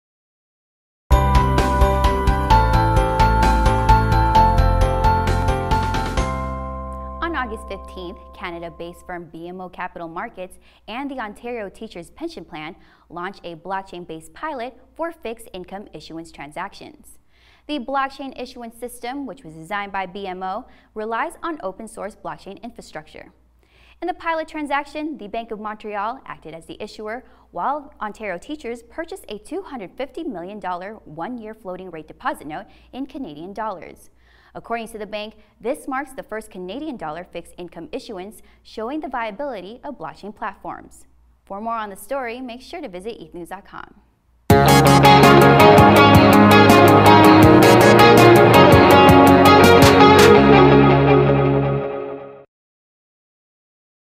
Bank of Montreal Looks To Blockchain For Fixed Income Issuance | ETHNews Brief
BMO hopes that Blockchain technology can help reduce the costs associated with fixed income markets.

ETHNews.com is the central hub for all things Ethereum.